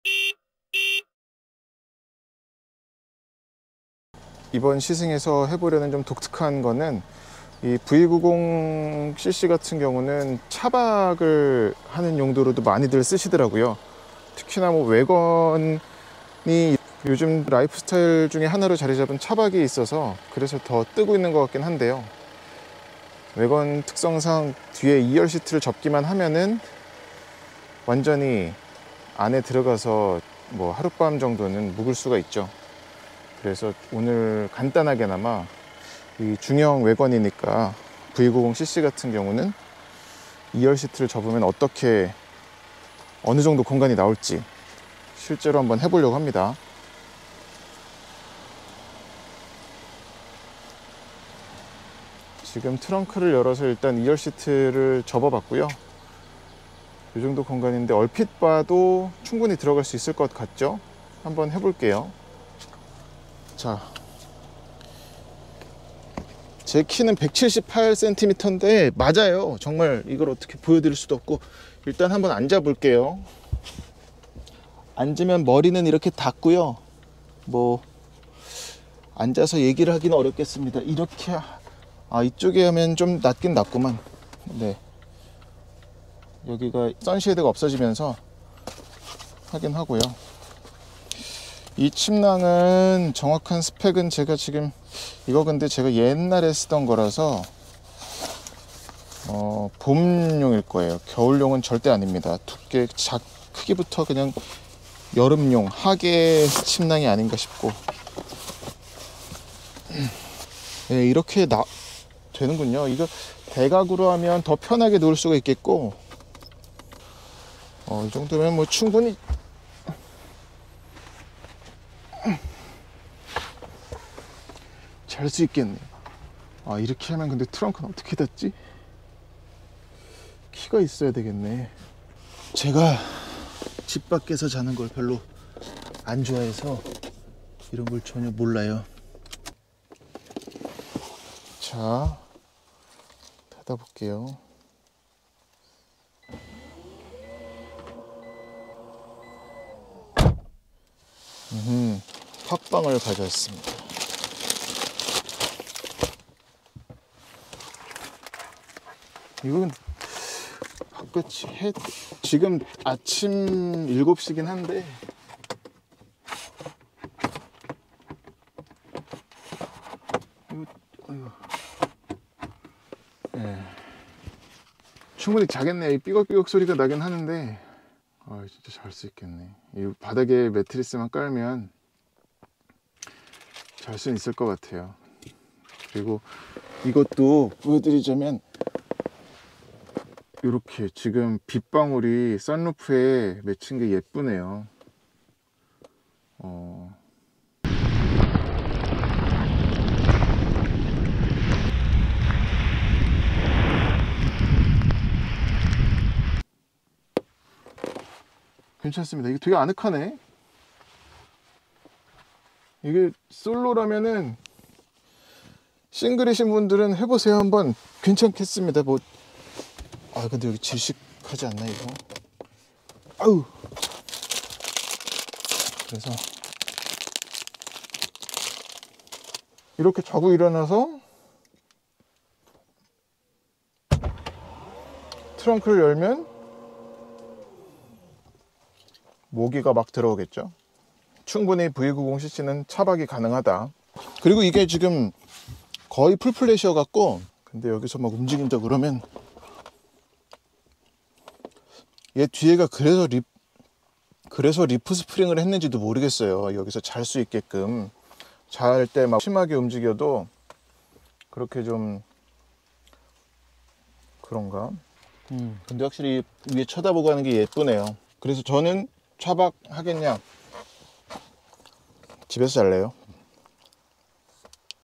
볼보 V90 CC에서 차박해볼까(One night at Volvo V90 Cross Country)
요즘 차박들 많이 하시죠? 지난 해 시승했던 볼보 V90 CC를 타며 실험해봤습니다. 충분히 눕겠네요. 전 캠핑은 1도 모르기 때문에 그냥 누울 수 있는지 누우면 어떤 느낌인지만 살폈습니다.